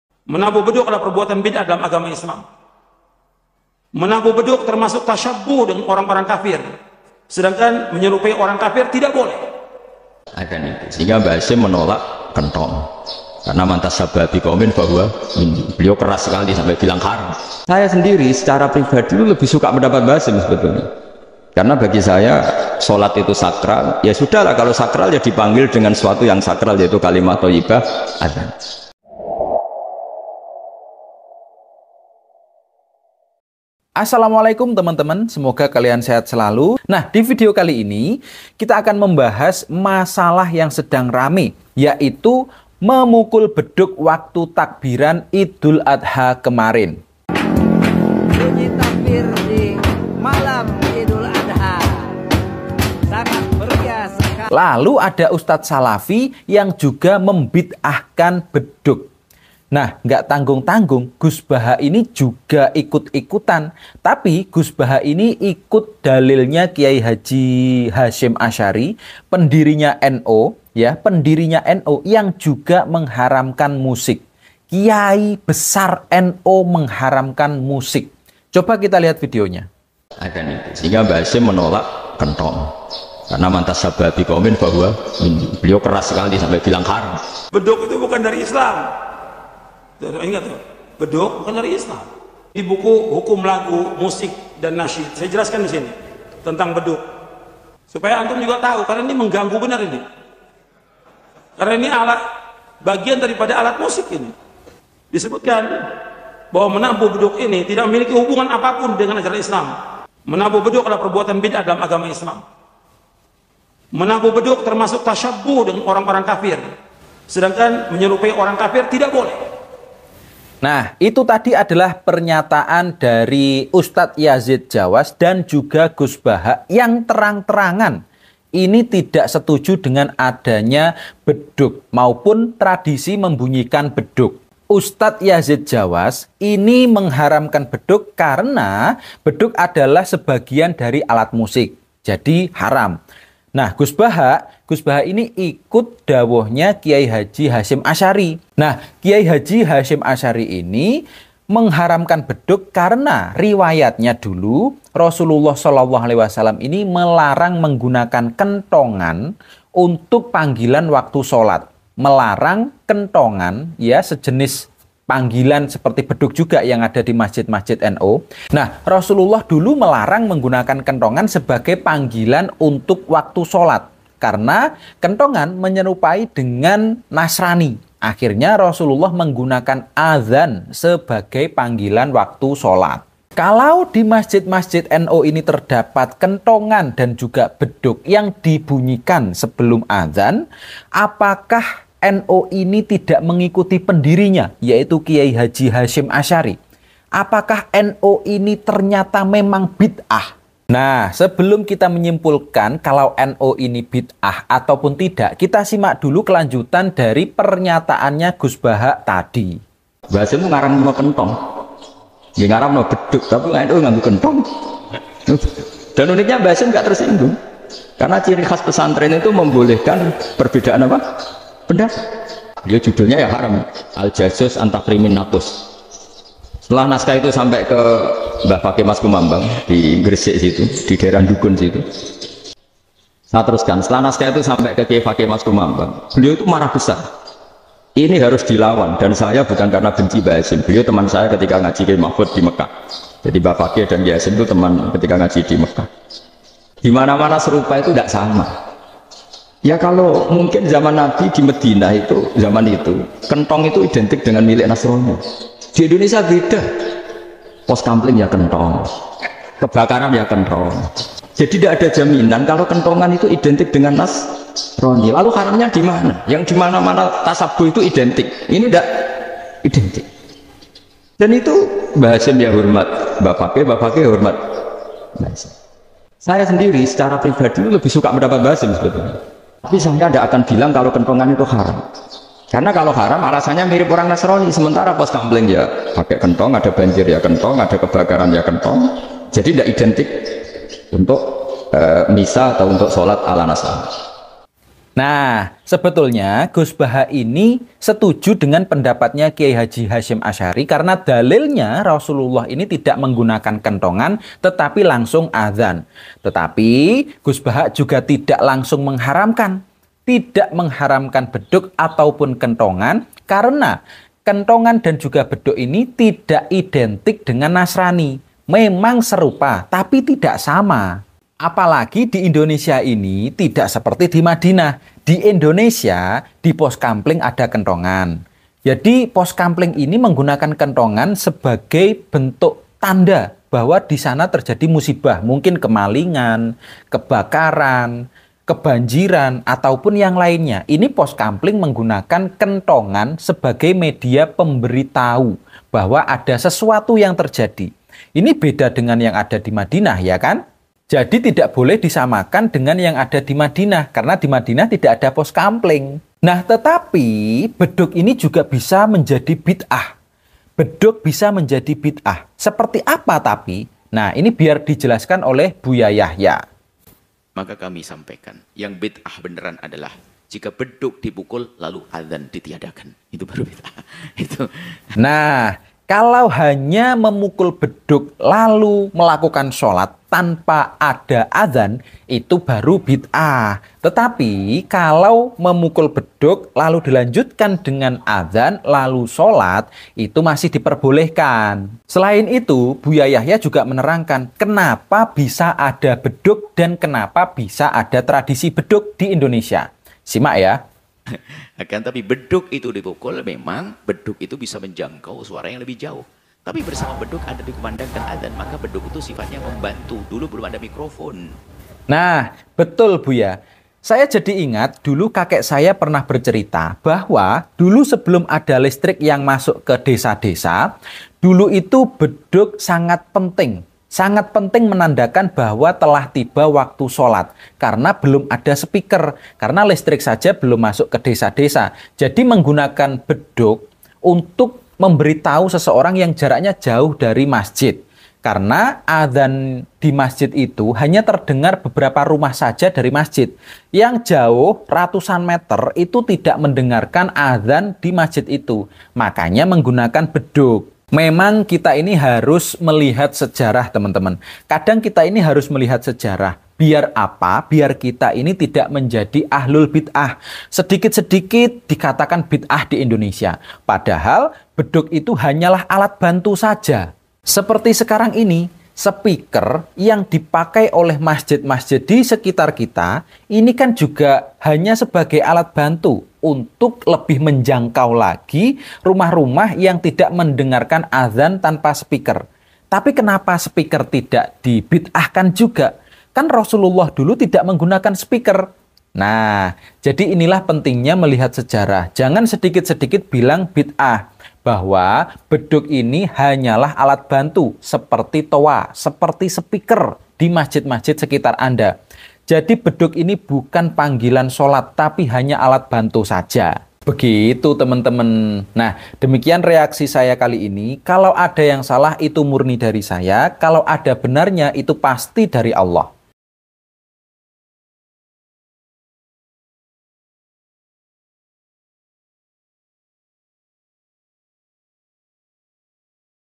TERBONGKAR..!! TERNYATA KYAI HASYIM ASY'ARI ITU WAHABI, begini tanggapan gus baha
ketika mbah hasyim ASY'ARI saja tidak menghalalkan bedug dan kentongan , bagaimana tanggapan gus baha ??